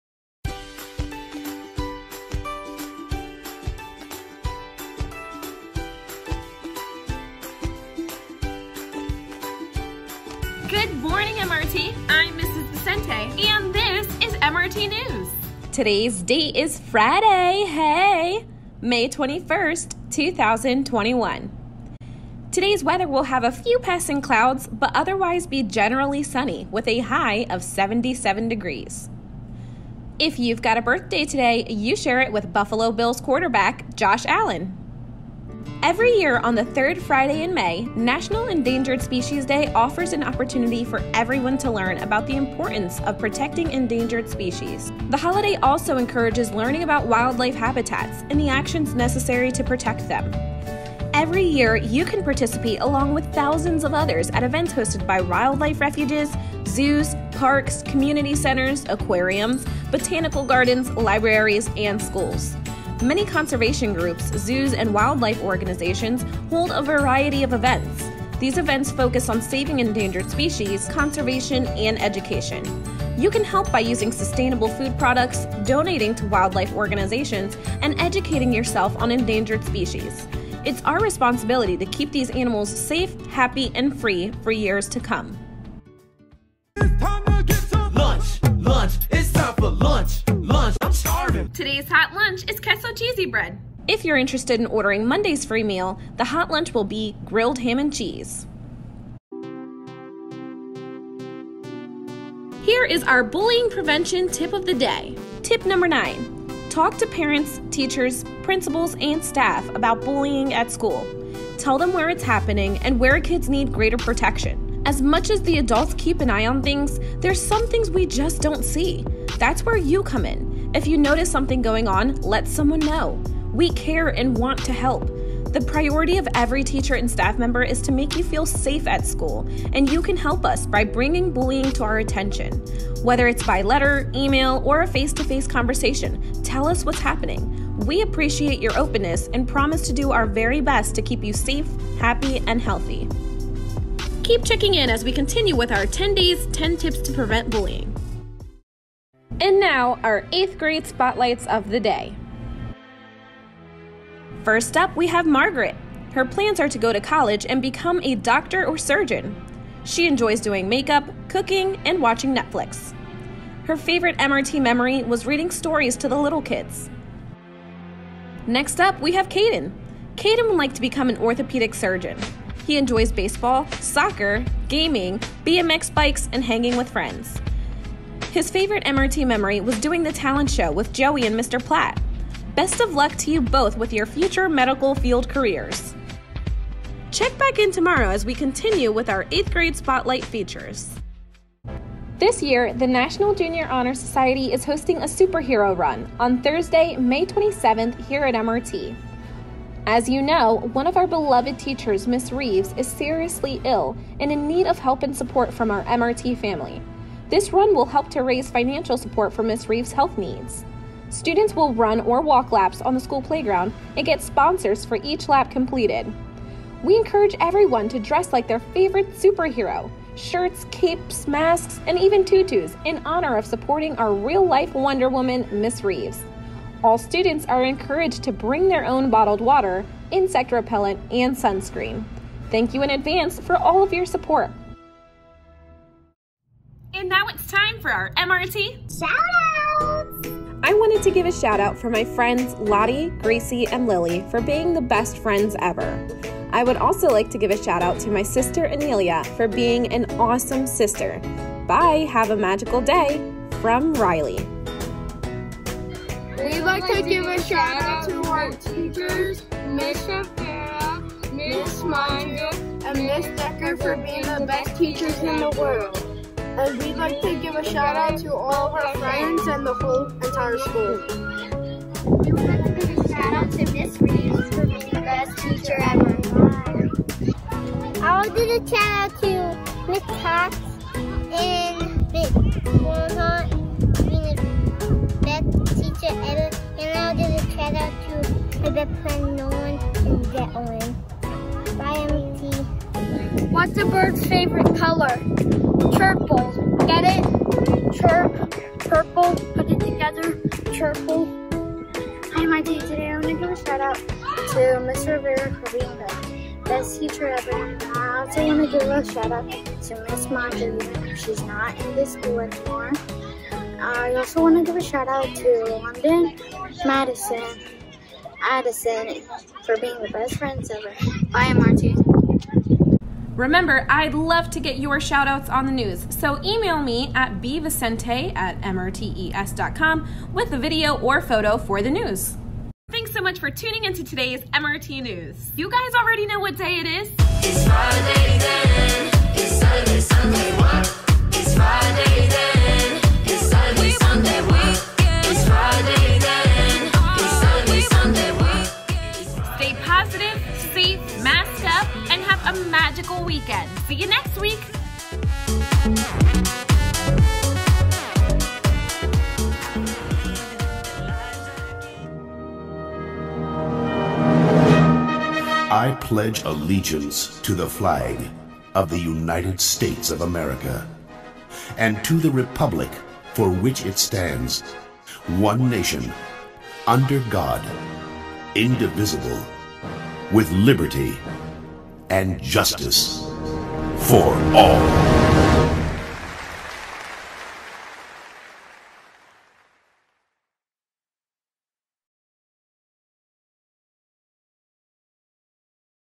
MRT News 5 21 21
National Endangered Species Day
Hot Lunch: Queso Cheesy Bread
Bullying Prevention 101
8th Grade Spotlight
Superhero Run
Shoutouts